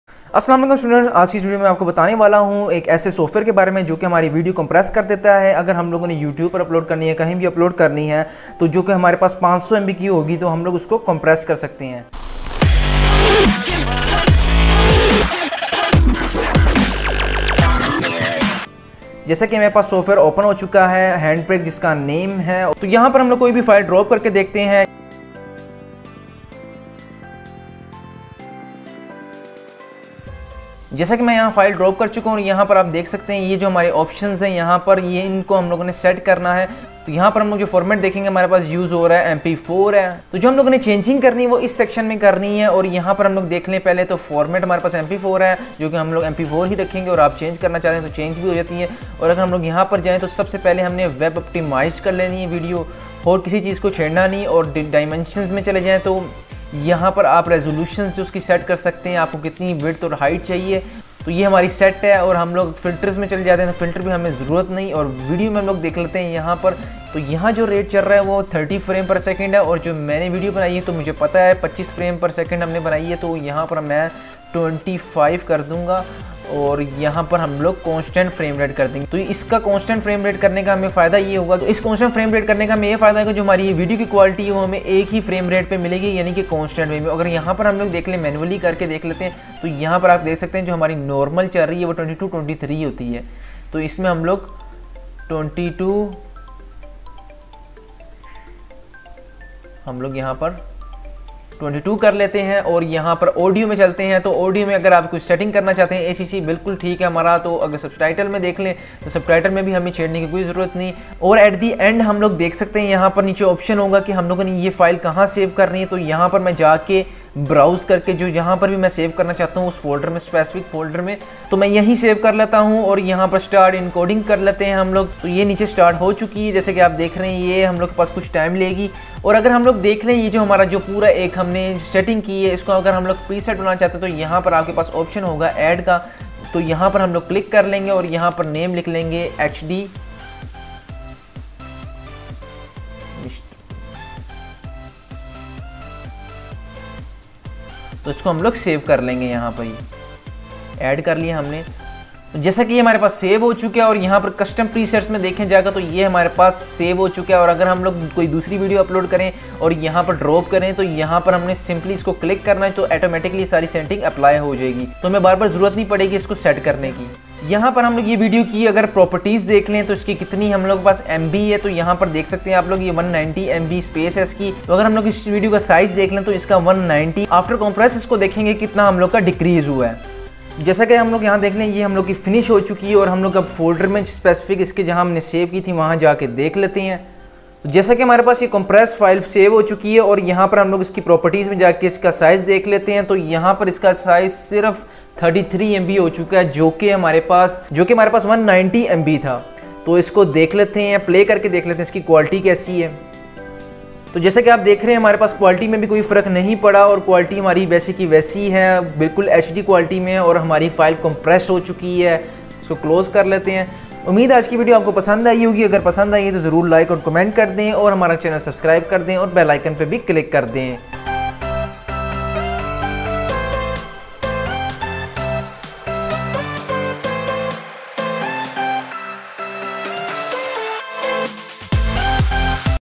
How to Compress Video without Loosing Quality
How To Use Handbrake Tutorial
The BEST Handbrake Settings & Beginners Guide  (2020)
Today we take a look at Handbrake, the open source video coder and compressor.